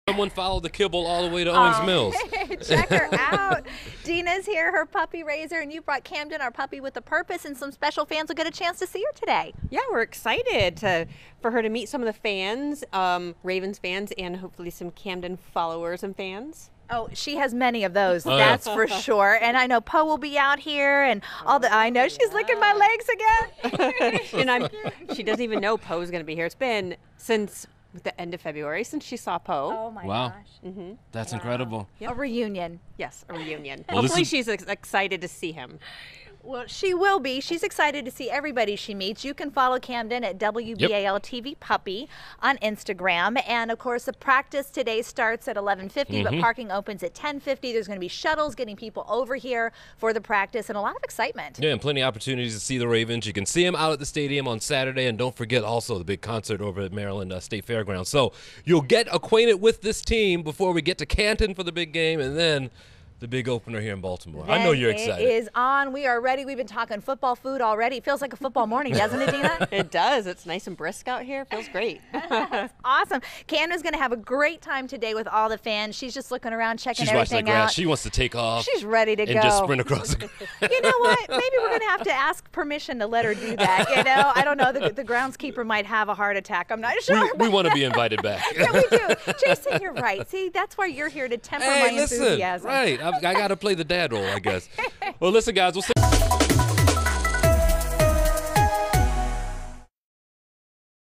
Camden's hanging out at Raven's training camp
WBAL-TV's Puppy with a Purpose is hanging out at Under Armour Performance Center in Owings Mills.